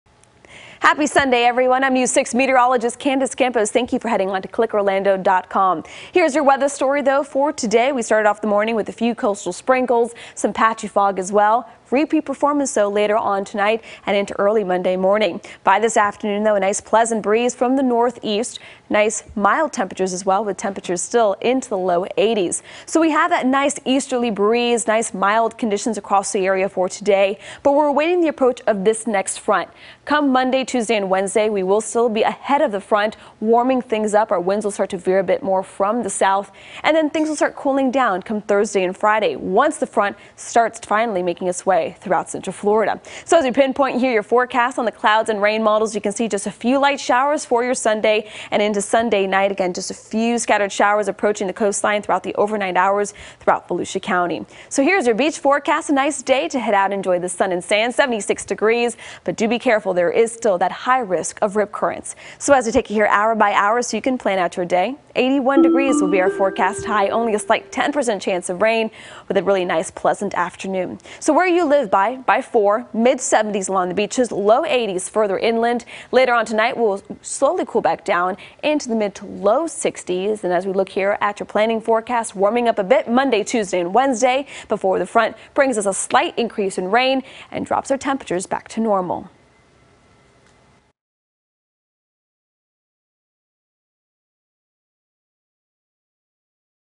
News 6 weather webcast
News 6 meteorologist Candace Campos' weather forecast for Orlando and Central Florida.